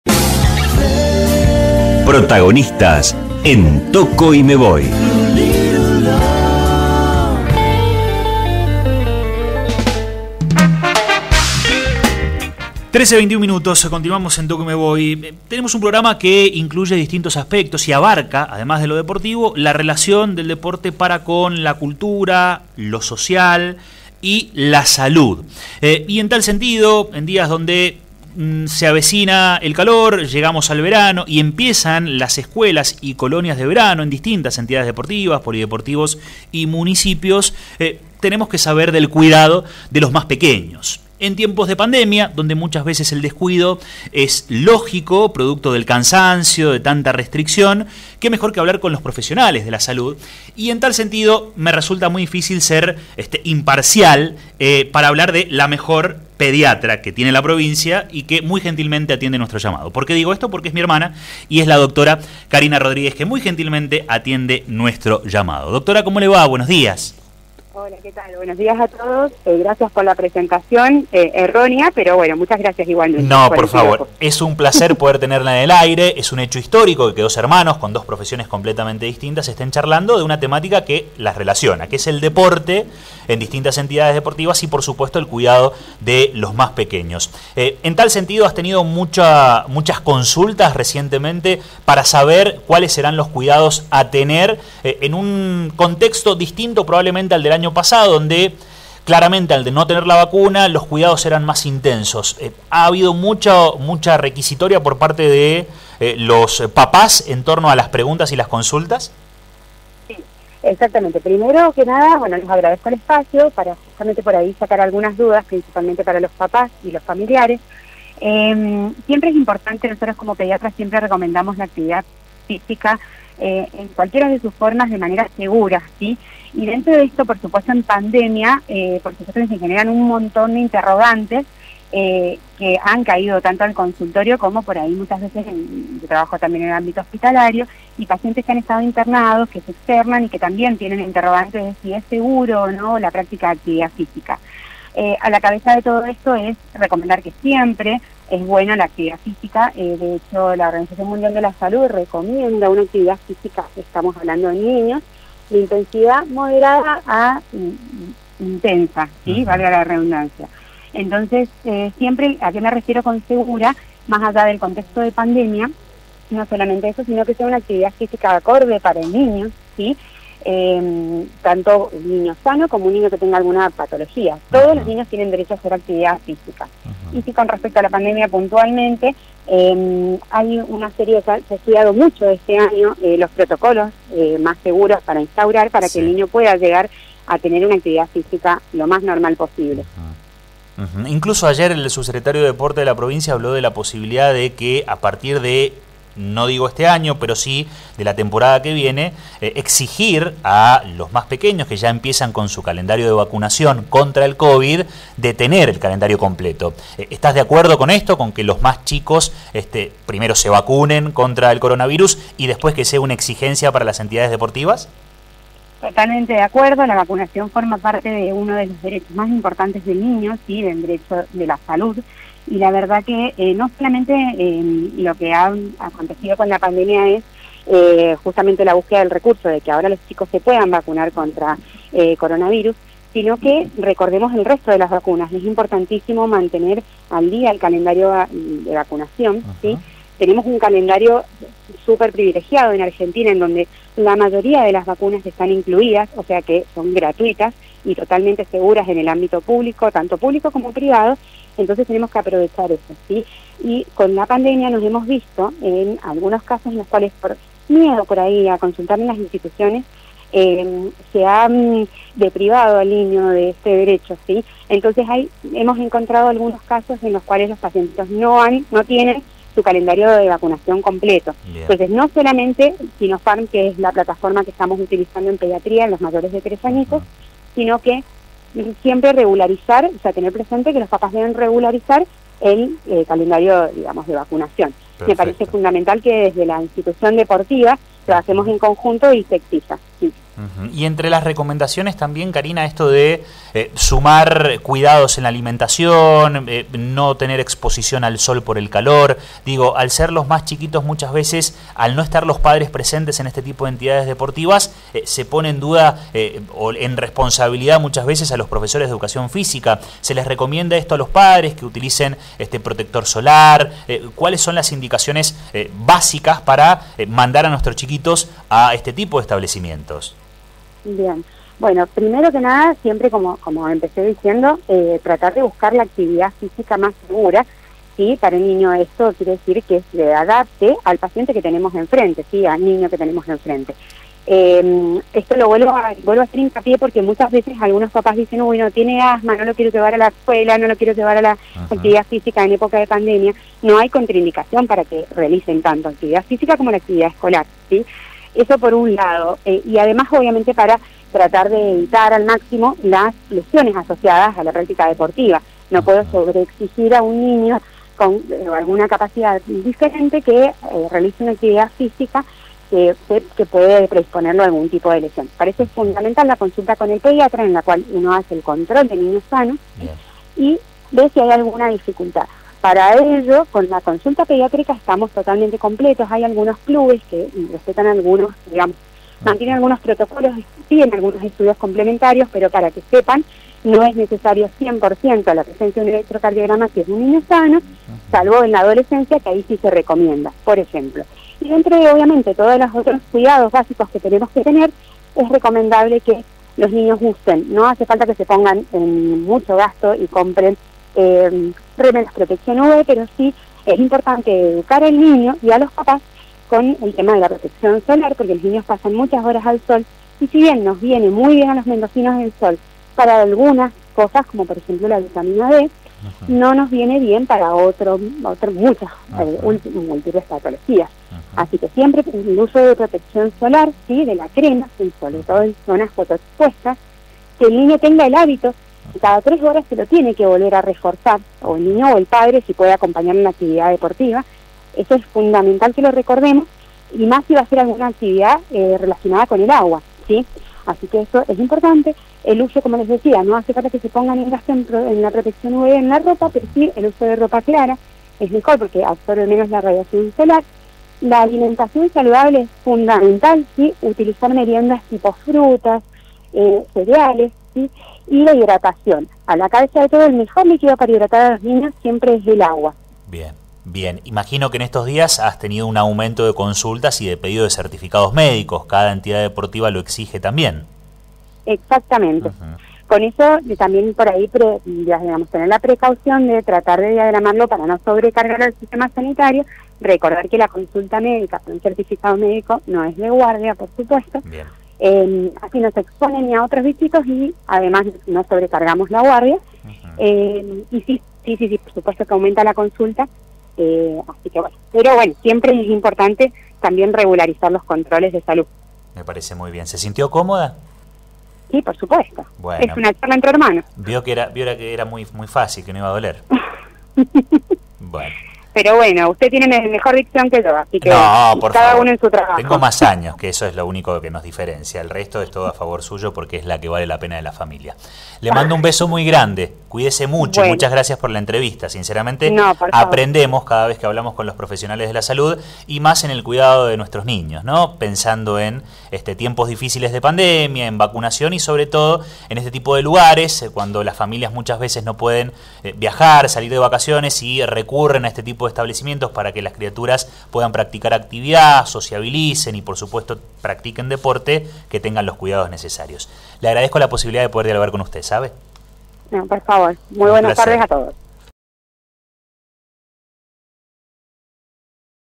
Karina Rodríguez - Pediatra - Escuelas de Verano
A días del inicio de las Escuelas y Colonias de Verano en Mendoza, dialogamos con la destacada Pediatra Karina Rodríguez, sobre el cuidado de nuestros niños y niñas. La alimentación, protectores solares, hidratación, controles médicos y vacunación; luego de que el Subsecretario de Deportes de la Provincia evalúe  junto a las federaciones deportivas, exigir en los establecimientos el calendario completo contra el Covid-19.